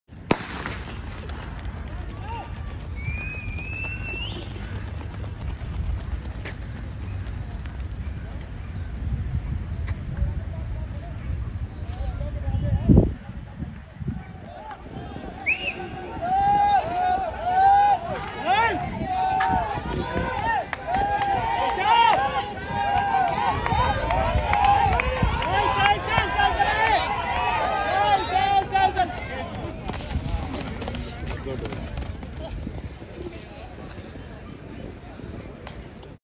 400mt boys final 82nd Delhi State Annual Athletics Championship 2022 at Jawaharlal Nehru stadium
The best 10 books you must buy to improve your state of mind :-

10 :- the wings of fire by apj Abdul Kalam :-